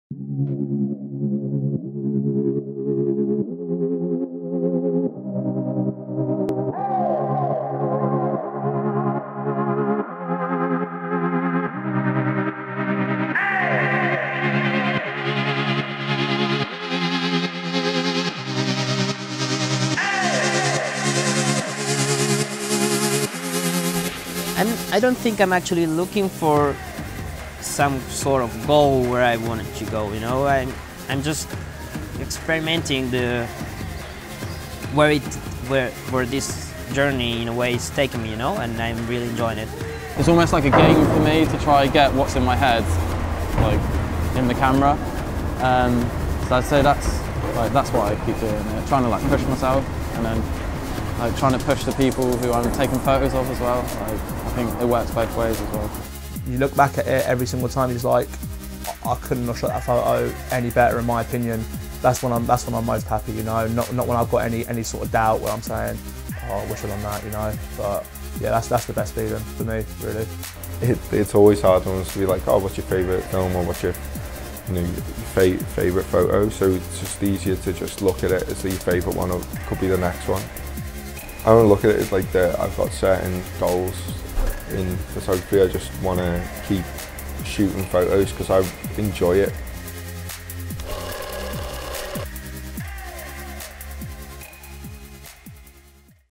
Frayed Issue 7 Trailer
Frayed Magazine is proud to present a series of film portraits with four of the best BMX & lifestyle photographers in the world.

We spent a day with each photographer as they shot photos with a BMX rider.

Featuring photographers Matty Lambert, Lucho Vidales, Jason Colledge & Eisa Bakos with riding from Ben Lewis, Mike Curley, Dan Boiski & Harry Mills-Wakely

Coming soon online & in print